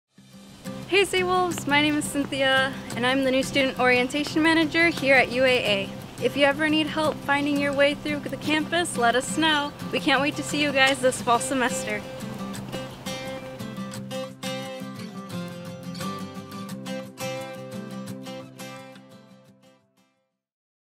Welcome back to campus from New Student Orientation Manager Cynthia Lopez
UAA New Student Orientation Manager Cynthia Lopez welcomes Seawolves back to campus for the fall 2021 semester.

(Note: At UAA, masks are not required outside as long as individuals are socially distanced. Visit uaa.alaska.edu/covid19 for updates on UAA's response to COVID-19.)